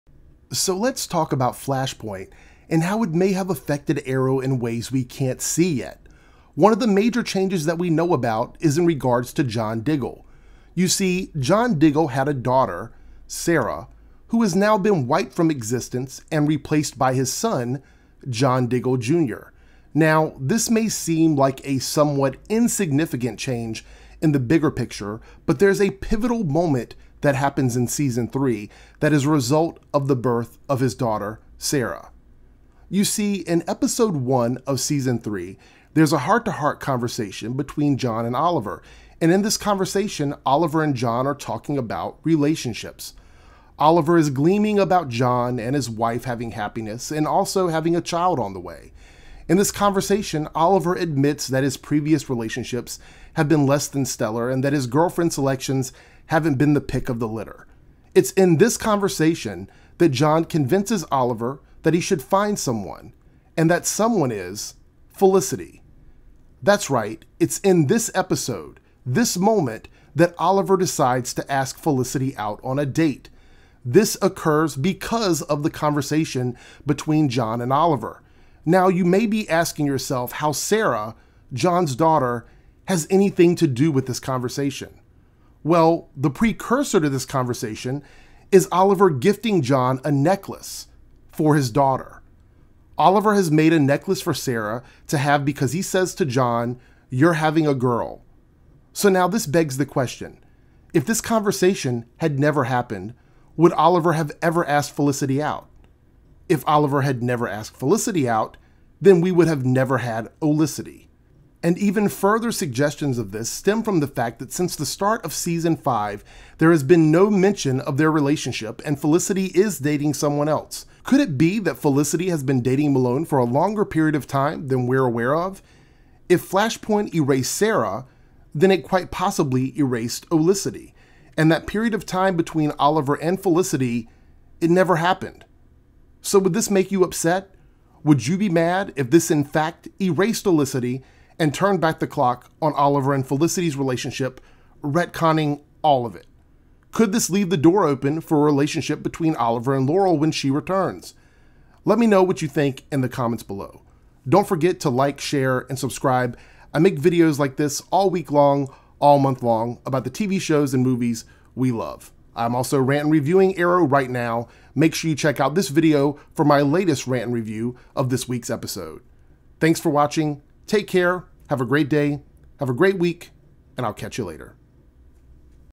Did Flashpoint Erase Olicity? - Lets Talk - Theories & Speculations!
It could have happened! Lets talk about how!